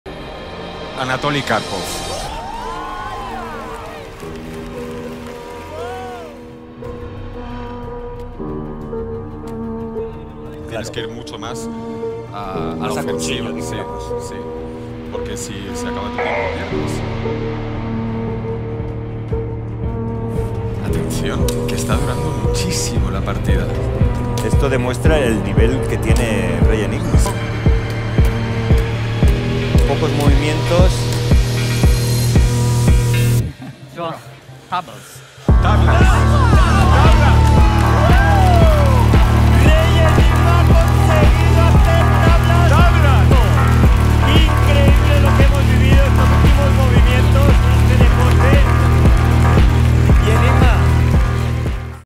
Maskeli Adam, Satranç Şampiyonu İle Maç Yapıyor
şampiyon terler içinde